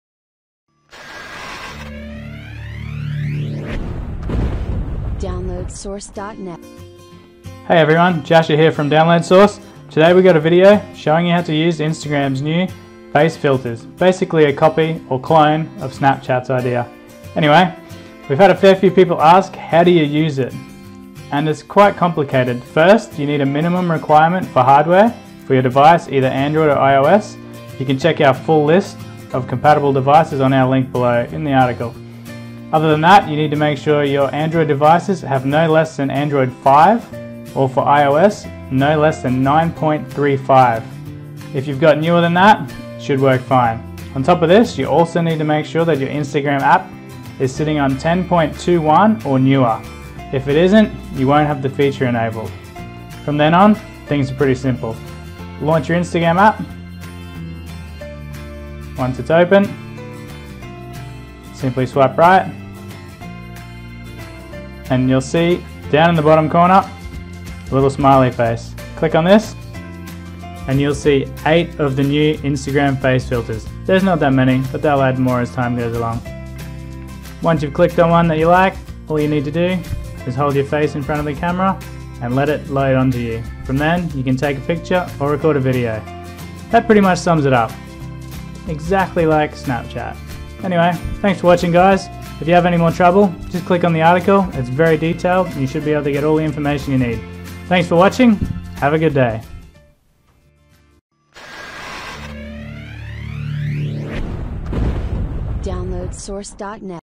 Trouble Shooting Instagram Face Filters (Compatible and Non-Compatible Devices)
If you are having any trouble getting the new Instagram Face/Selfie Filters working on your device this guide will shed some light on the situation.